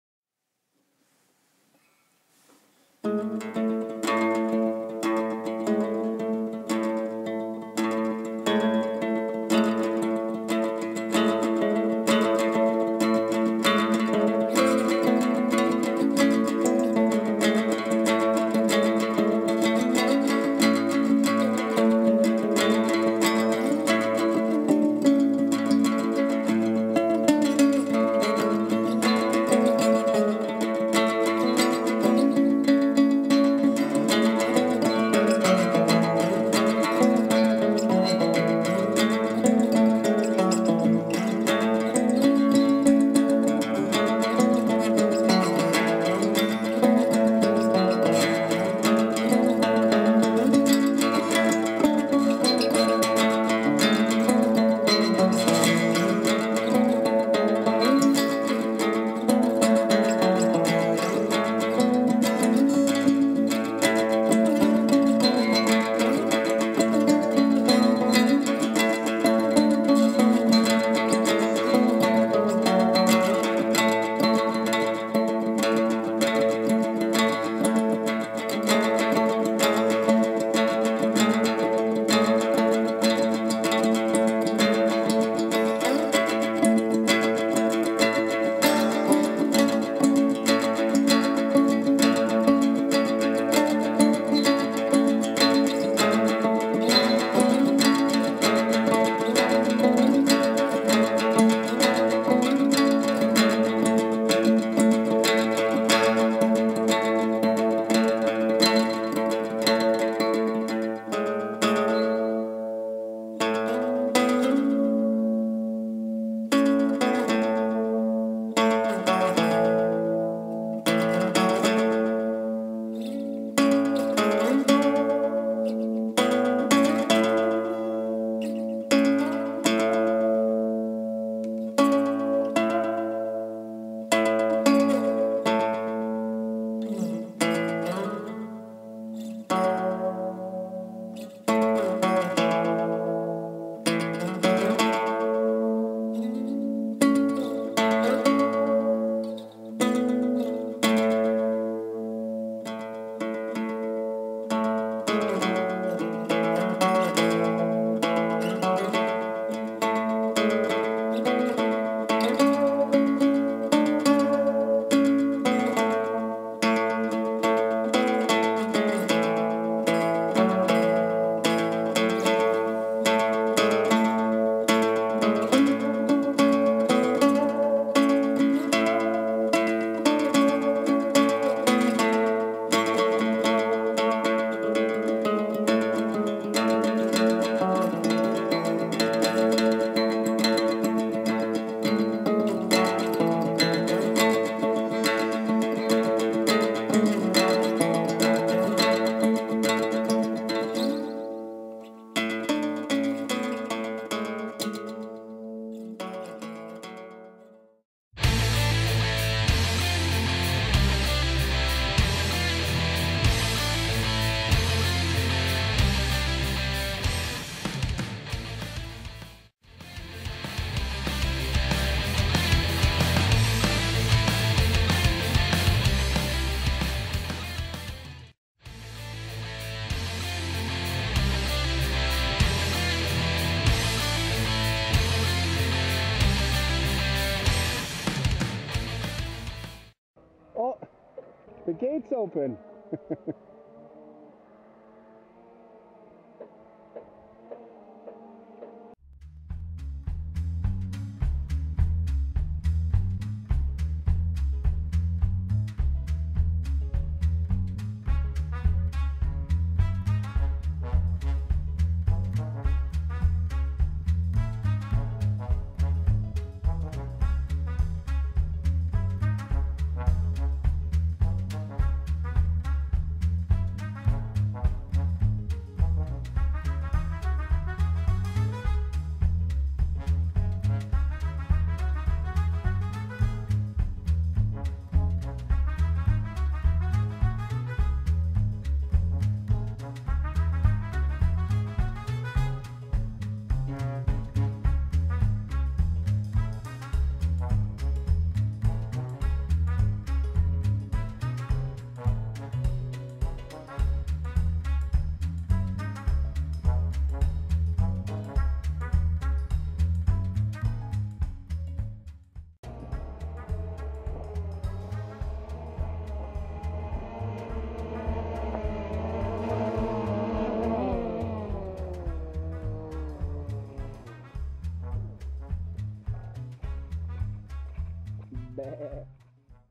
Drone herding rams from pen to pen to field to pen
These rams didn't want to move, little persuasion and they can be moved anywhere.  Dog retirement day.